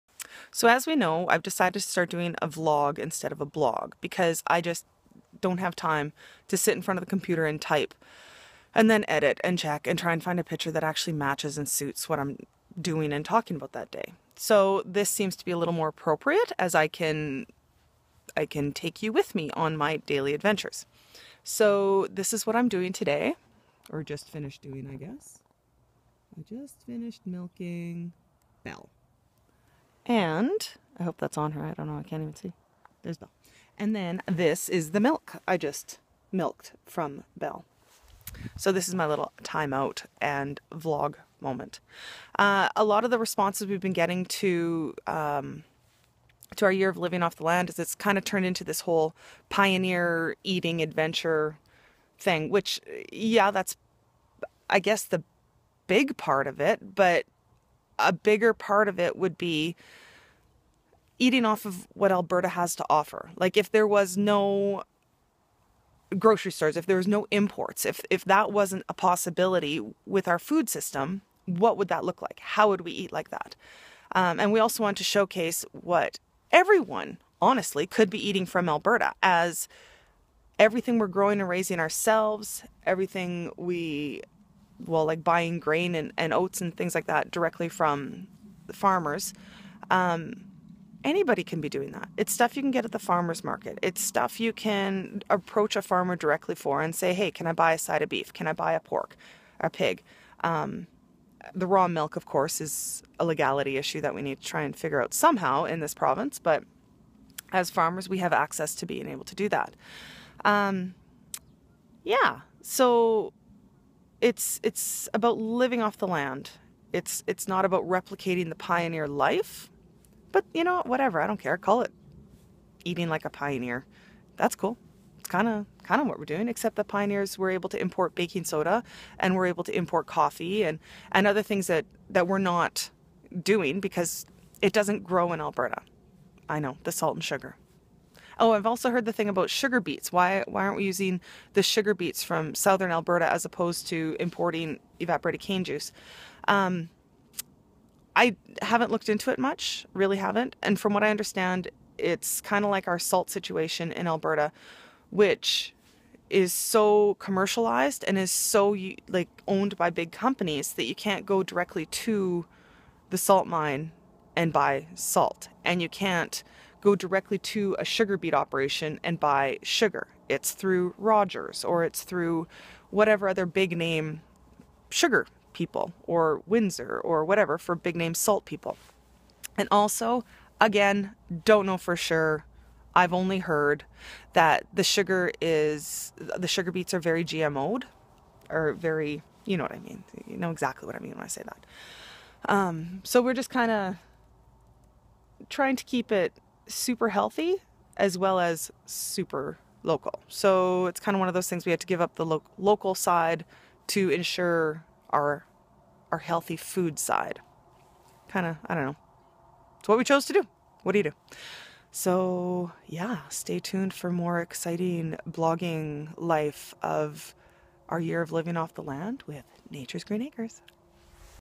Our Year of Living off the Land
a few of the thoughts and comments I have about our Year of Living Off The Land.